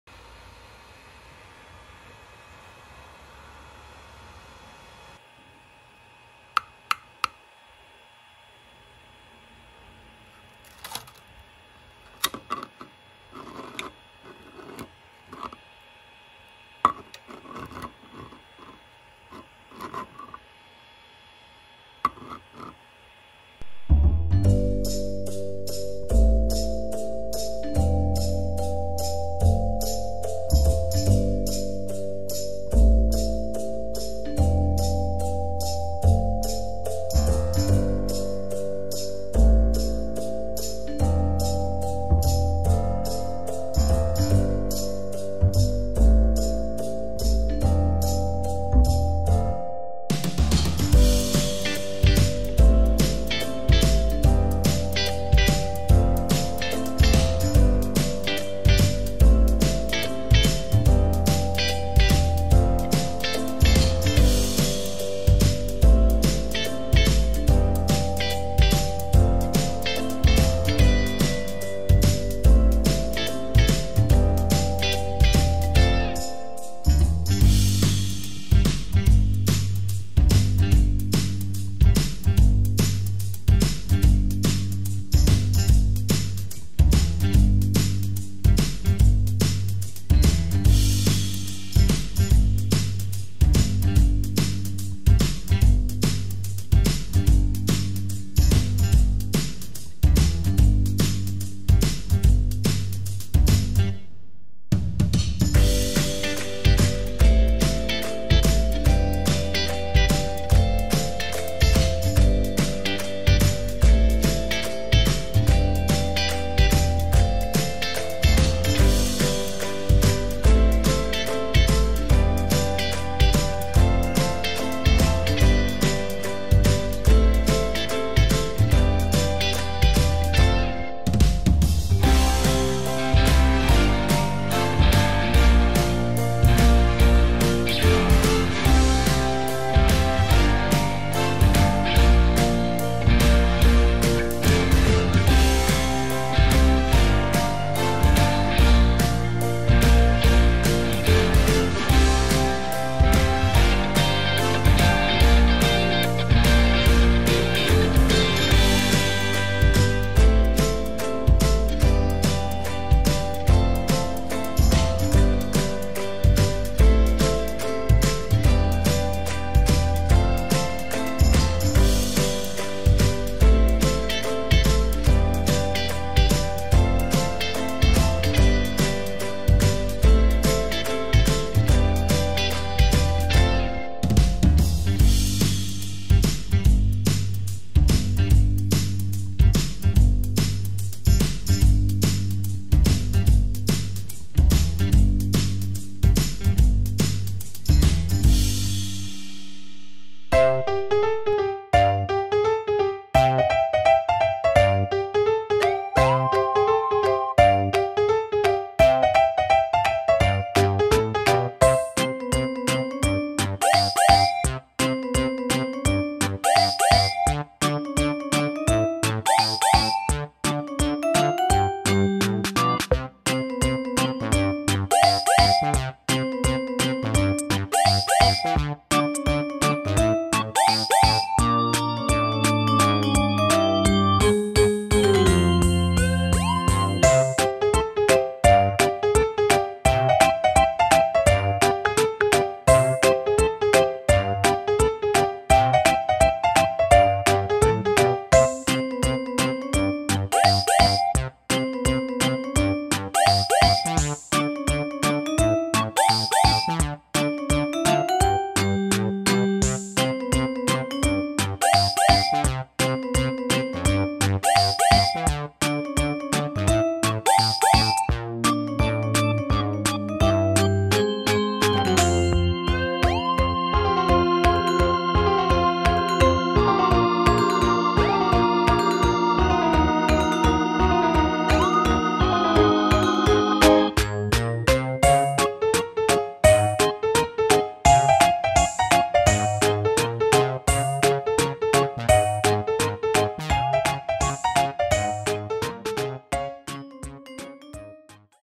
109) Boxy Squirrel Box with Tinkercad + 3D printing | 3D modeling how to make
Using only boxes for boxy Squirrel box :D